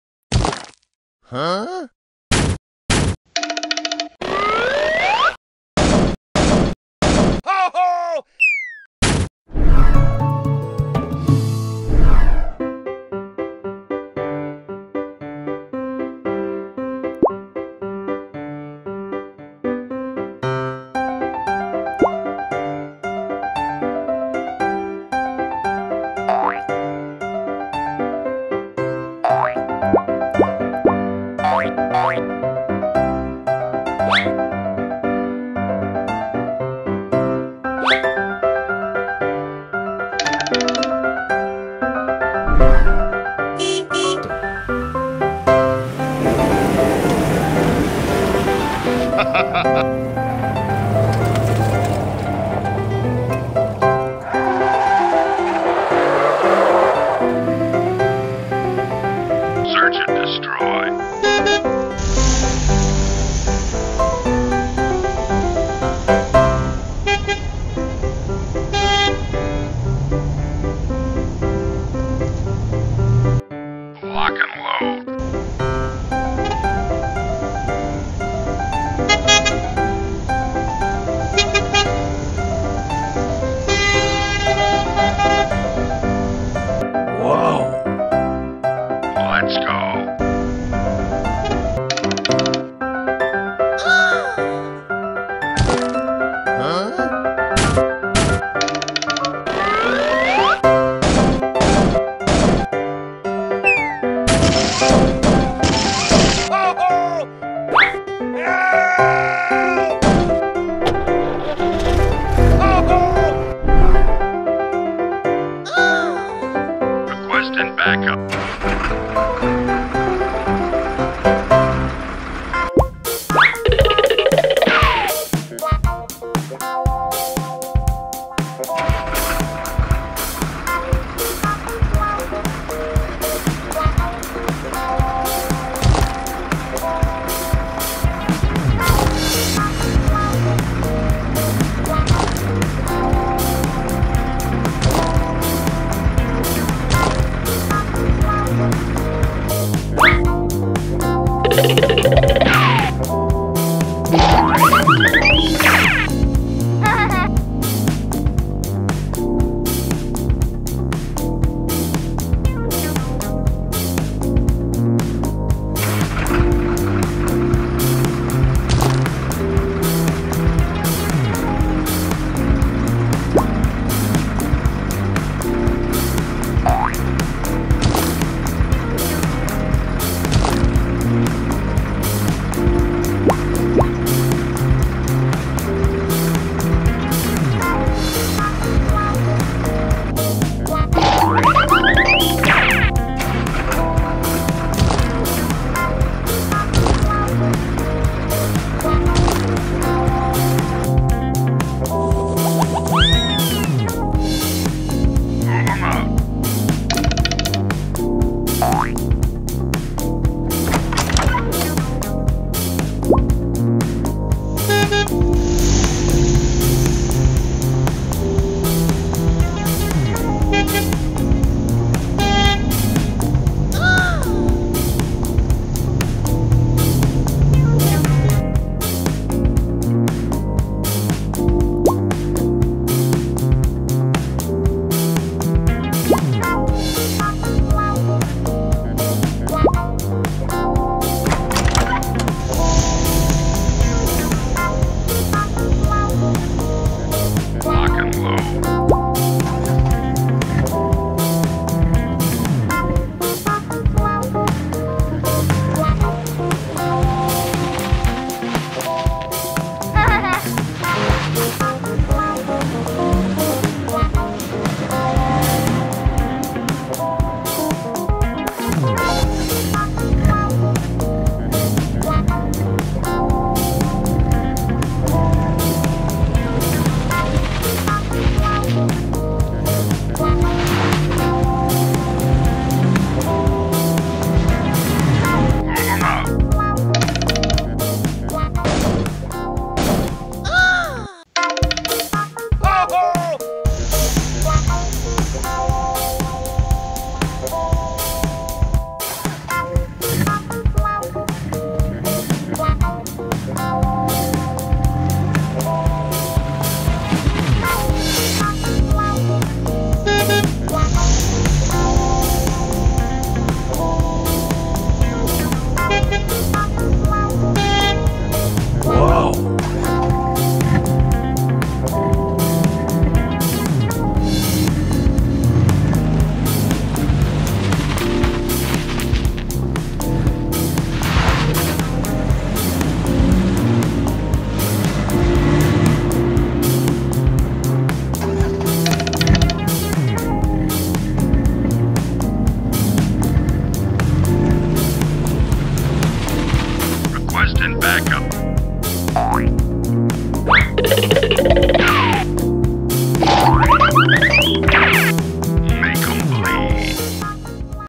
Automobile toys！！The truck is stuck in the sand. Let's rescue it.Kid toy video丨CW Toys TV
Four new piles of sand were piled up, and the vehicles were passing through the middle of the piles. But the truck came and was too big and stuck in the middle of the two piles. It cried out for help. Finally, the excavator and the bulldozer came to rescue it.